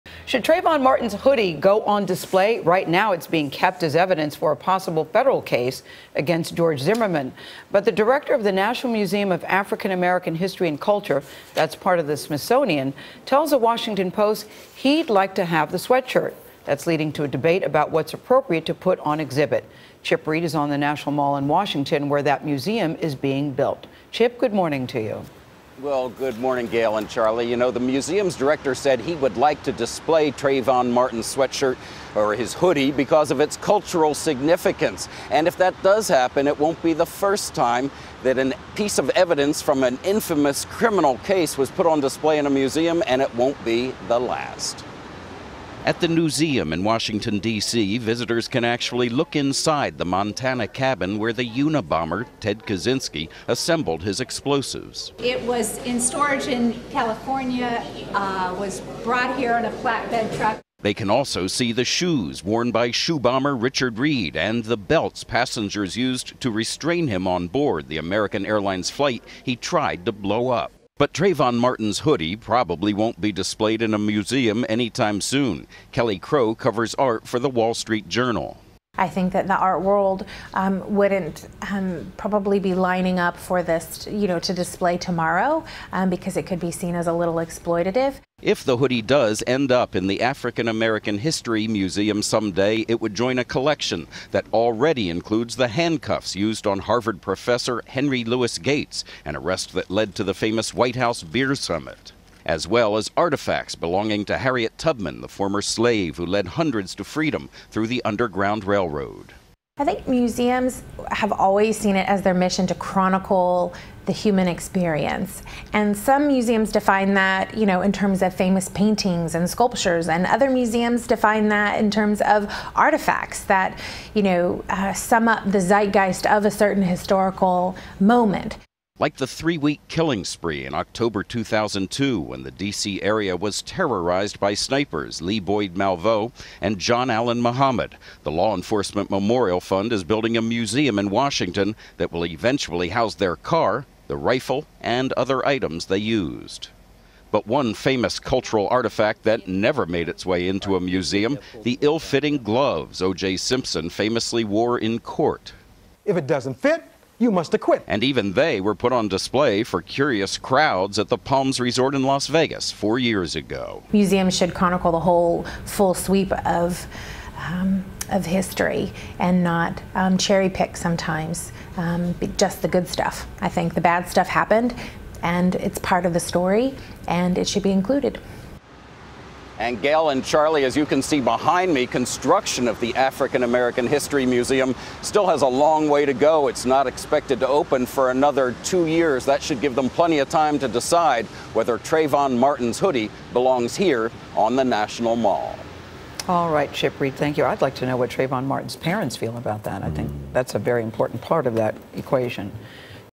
Trayvon Martin's hoodie: Should museums acquire crime-related items?
Trayvon Martin's hoodie is being kept as evidence, but a Smithsonian director told the Washington Post that he is interested in displaying the sweatshirt, in a similar fashion to exhibits featuring other crime-related items. Chip Reid reports.